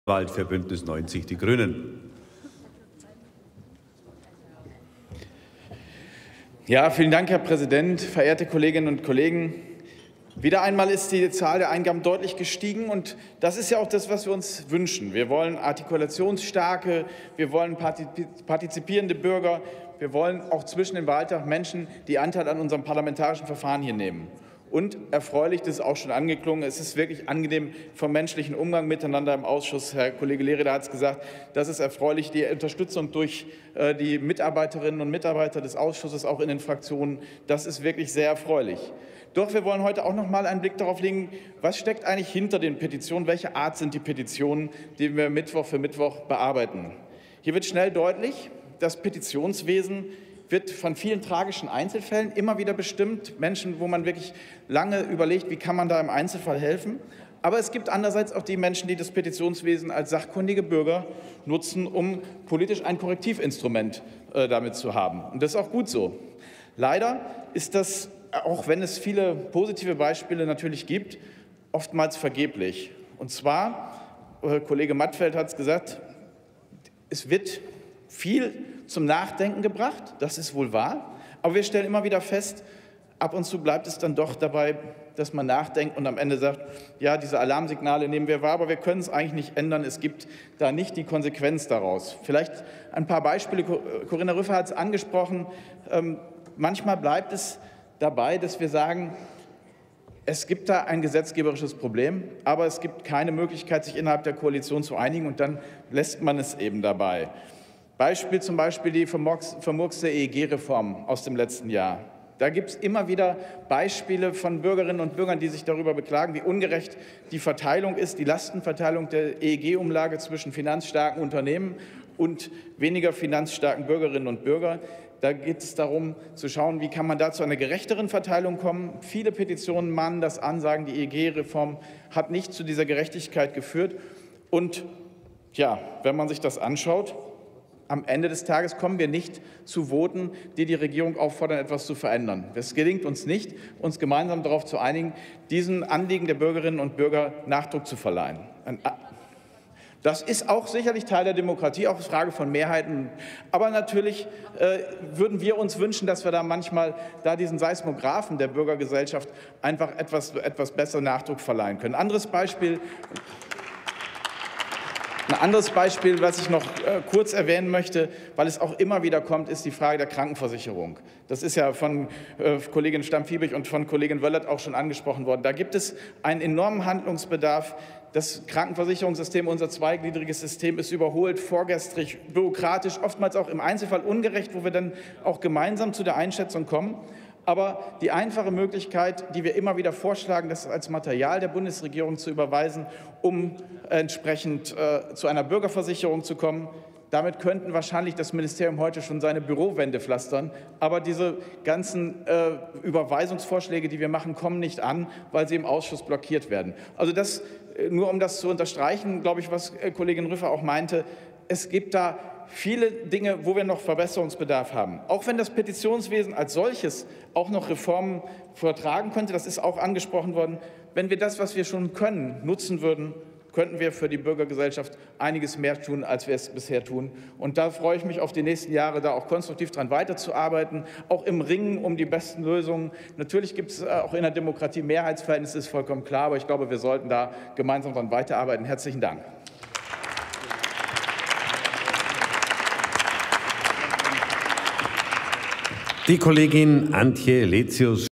Peter Meiwald: Tätigkeitsbericht 2014 des Petitionsausschusses [Bundestag 11.06.2015]
Beratung des Berichts des Petitionsausschusses (2. Ausschuss)
Bitten und Beschwerden an den Deutschen Bundestag
Die Tätigkeit des Petitionsausschusses des Deutschen Bundestages im Jahr 2014
Drucksache 18/4990

18. Deutscher Bundestag
109. Plenarsitzung
Tagesordnungspunkt: Tätigkeitsbericht 2014 des Petitionsausschusses
Redner: Meiwald, Peter, B90/Grüne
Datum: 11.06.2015
Plenarprotokoll:
UVID:5228091